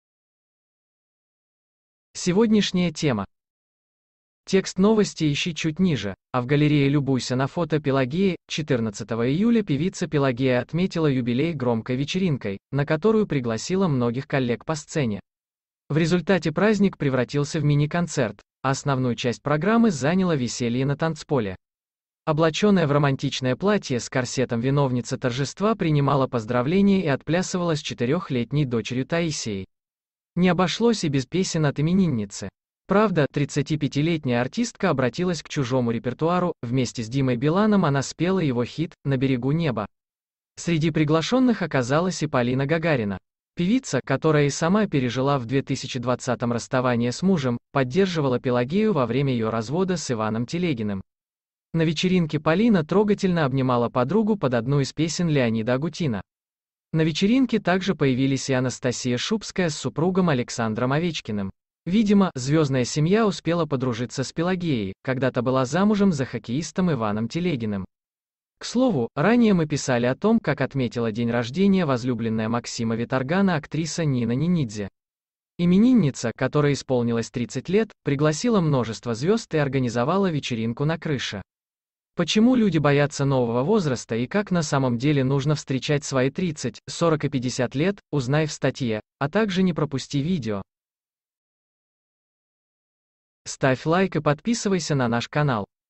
Дуэт с Биланом, танцы с дочерью, Овечкины в гостях: как Пелагея отметила 35-летие
Певица устроила роскошную звездную вечеринку.